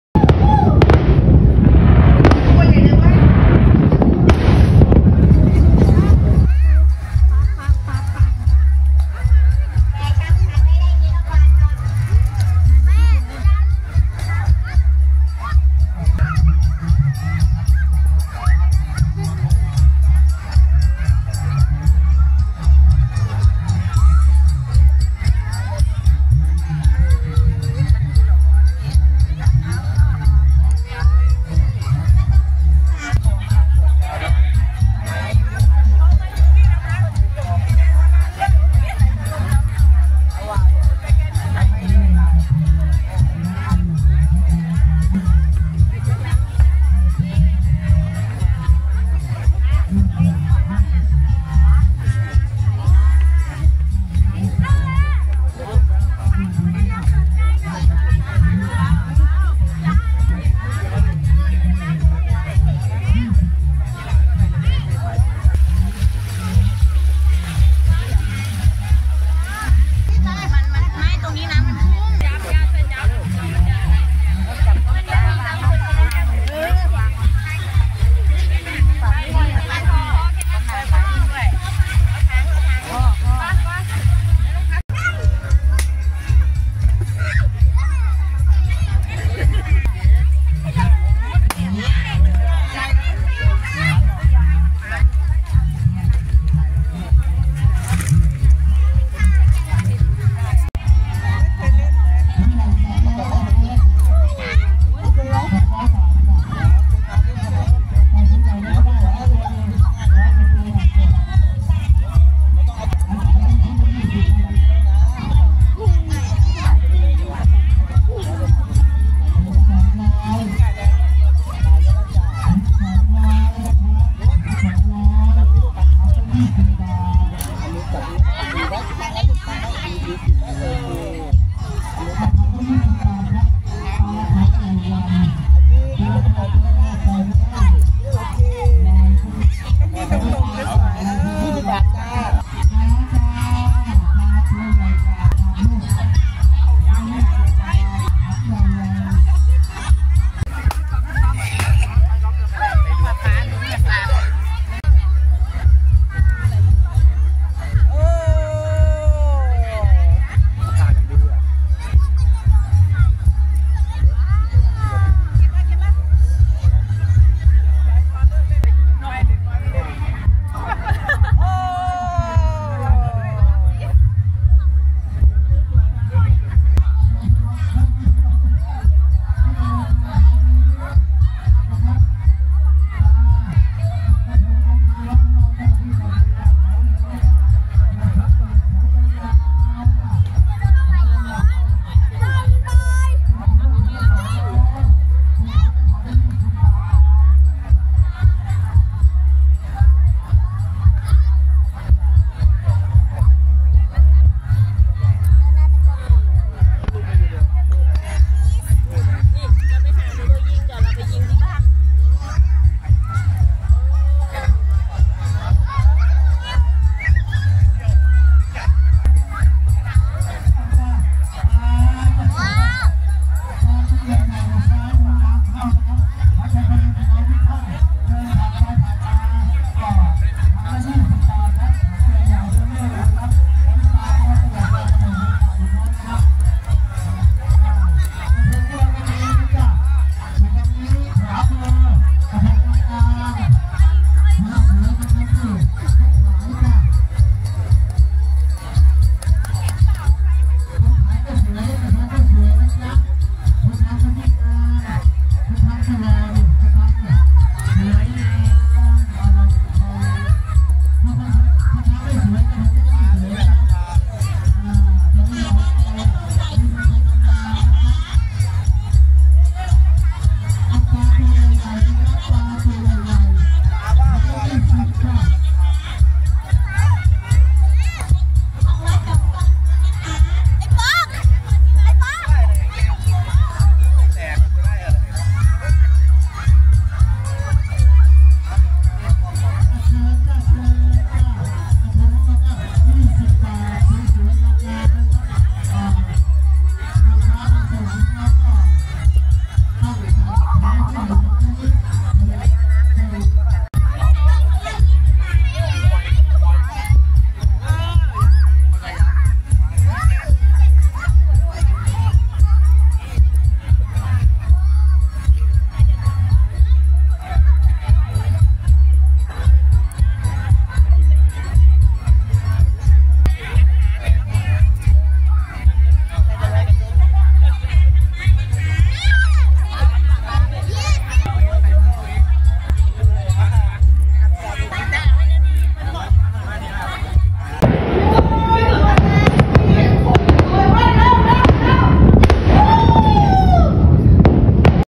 ลอยกระทงกับน้องชามา
ประเพณีไทยลอยกระทง ปี 2566 เทศบาลตำบลโพธิ์ดลาง เมืองนครราชสีมา